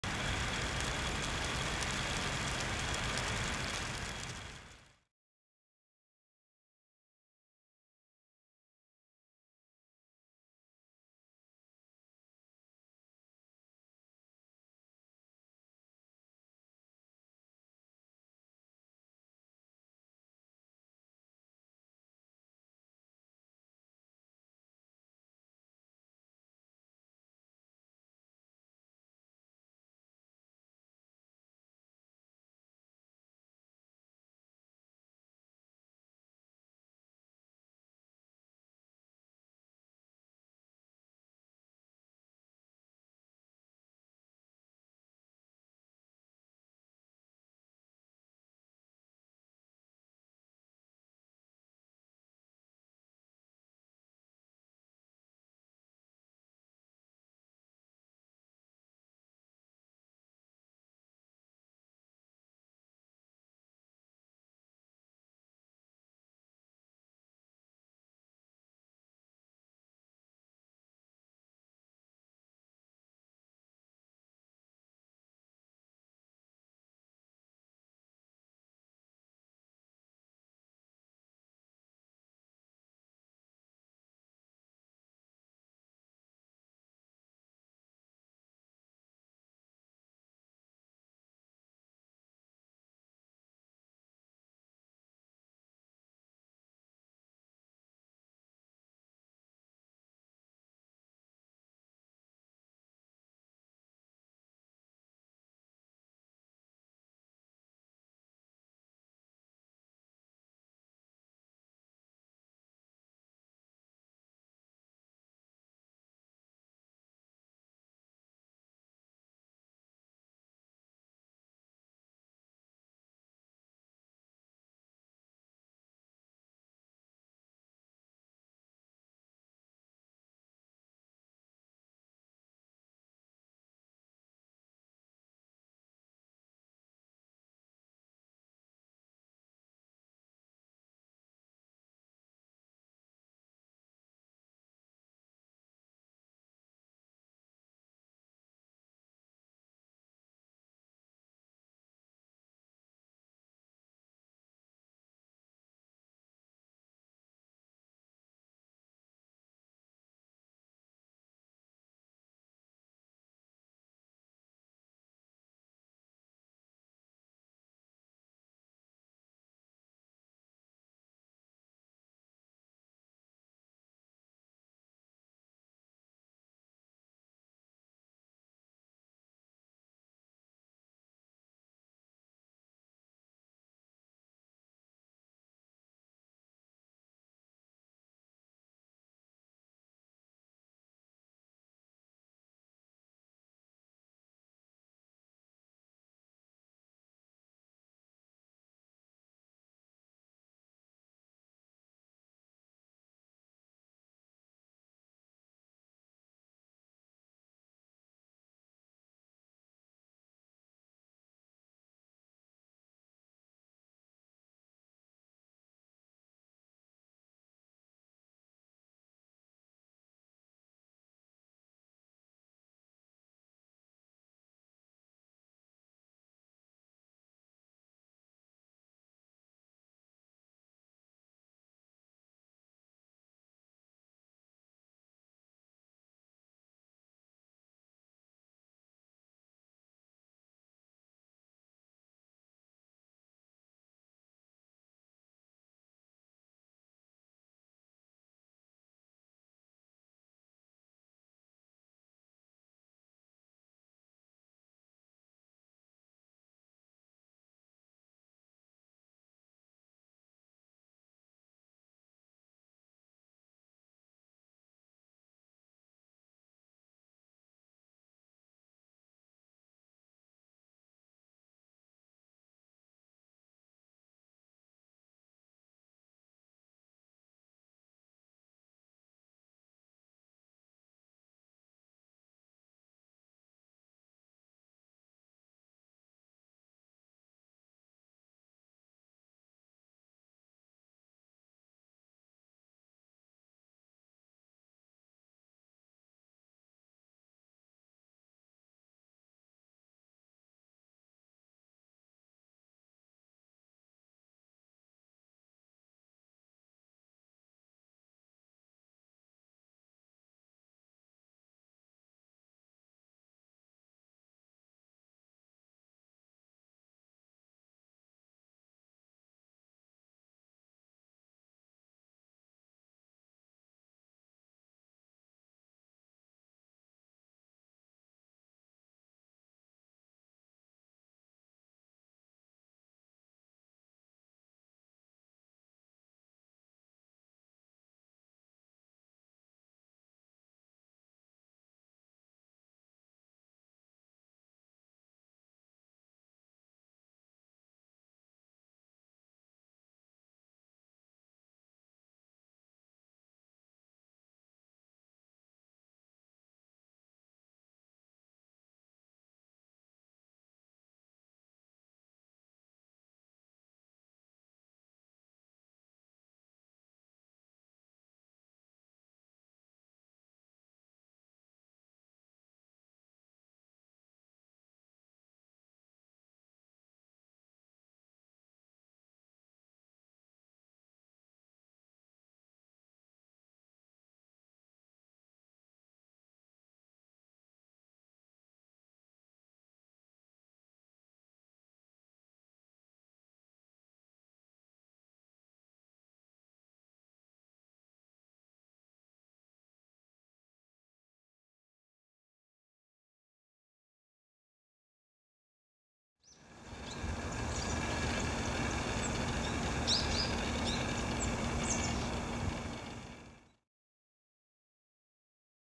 2015,서울 첫눈 슬라이드쇼 -Yookeun Chang
서울에 꿈같은 첫눈이 내렸습니다.